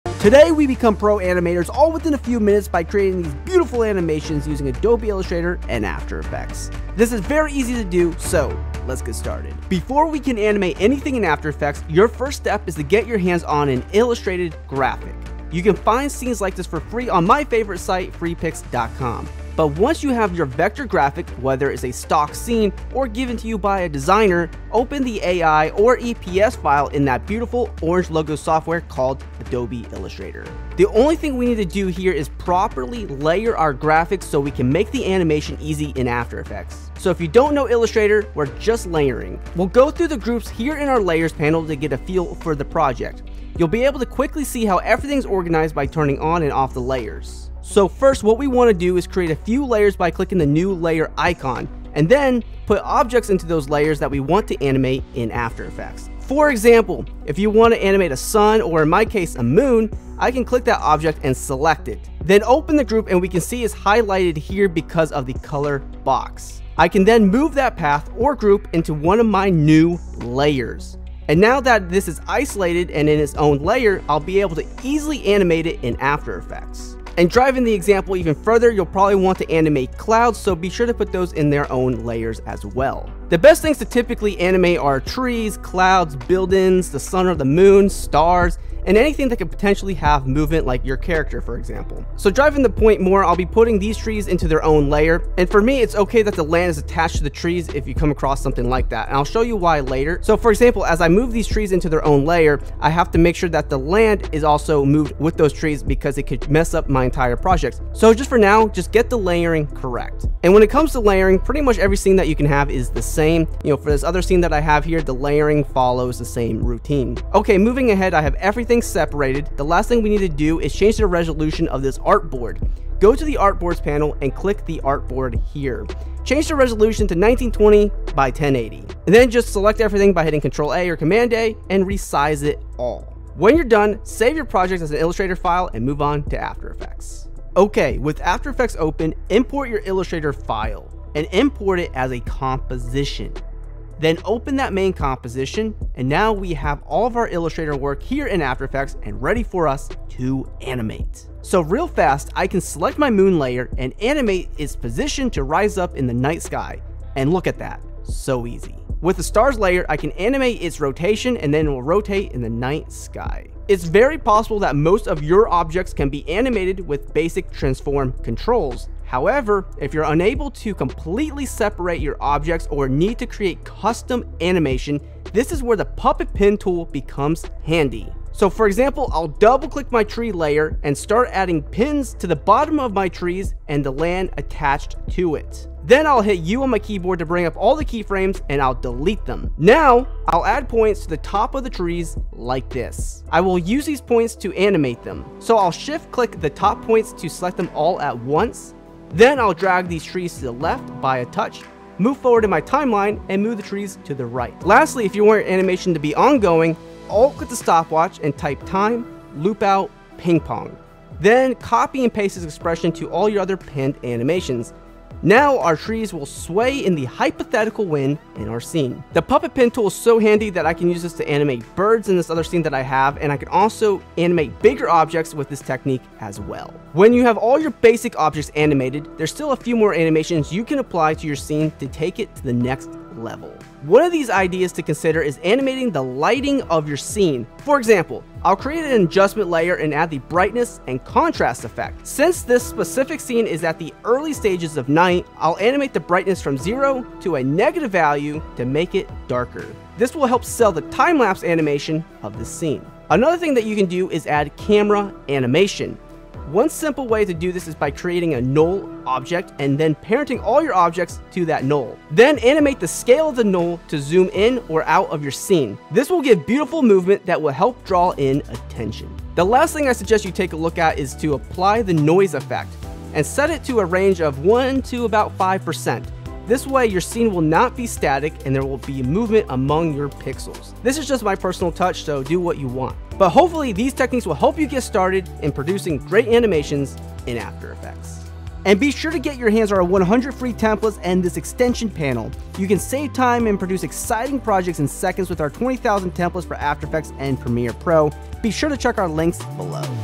Create Animated Videos in After Effects
20,000+ templates for After Effects - Become a pro animator in After Effects by creating animated videos. In tutorials uses an Adobe Illustrator to After Effects workflow to animate entire scenes. We'll be focused on animating essential objects that you will likely animate in your After Effects animated videos.

Timecode:
0:00 Intro - Smash that like button!
0:10 Step 1 - Adobe Illustrator Laying
2:17 Step 2 - After Effects Animation